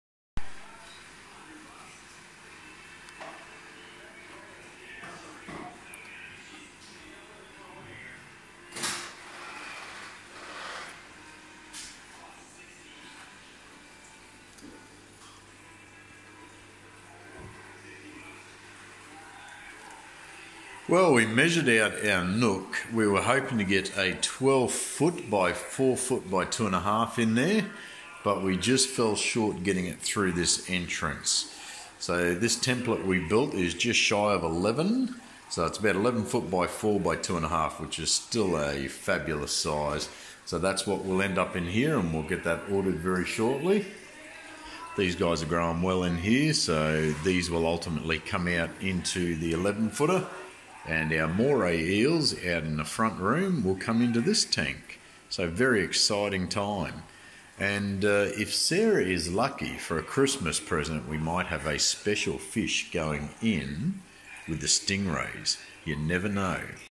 Large stingray tank template
Well we were hoping for a 12x4x2.5
But just fell short on the entry to the nook
So it looks like we will settle for the 11x4x2.5 weapon
Ordering soon